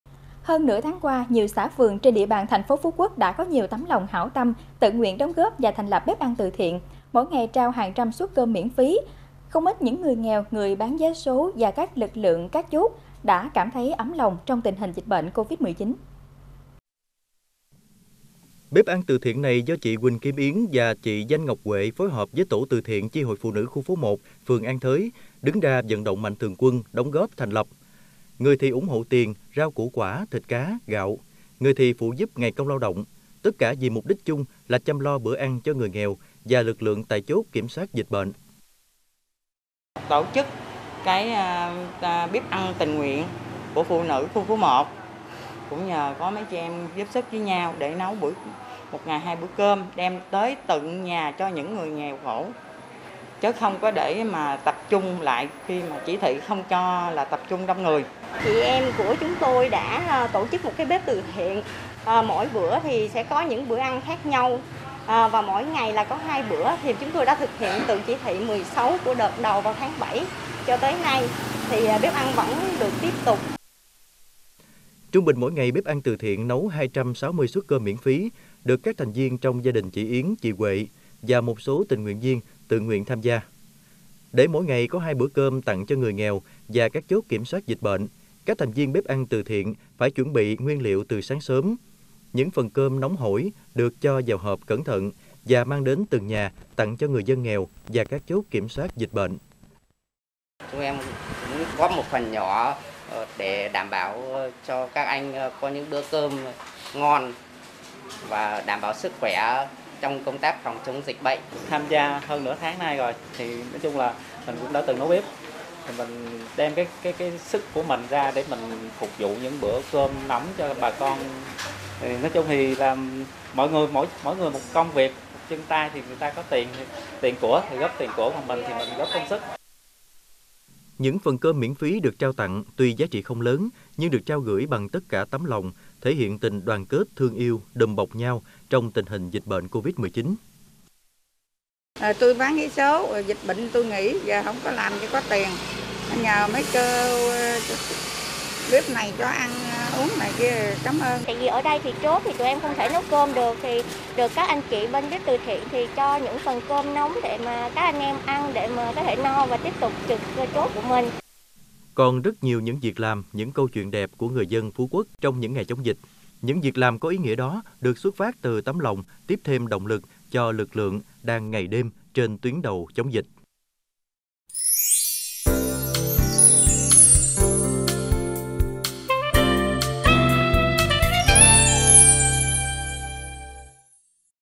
Phú Quốc lan tỏa tấm lòng nhân ái trong đại dịch Covid-19 | THKG
Hơn nữa tháng qua, nhiều xã, phường trên địa bàn thành phố Phú Quốc, đã có nhiều tấm lòng hảo tâm tự nguyện đóng góp và thành lập bếp ăn từ thiện, mỗi ngày trao hàng trăm suất cơm miễn phí, giúp không ít những người nghèo, người bán vé số và các lực lượng trực các chốt để họ cảm thấy ấm lòng trong tình hình dịch bệnh  Covid-19.